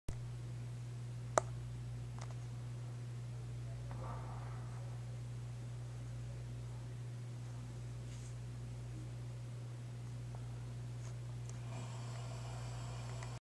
Freaky Tiki's in bathroom at the Polynesian Resort
I was scared they would come to life and kill me and my family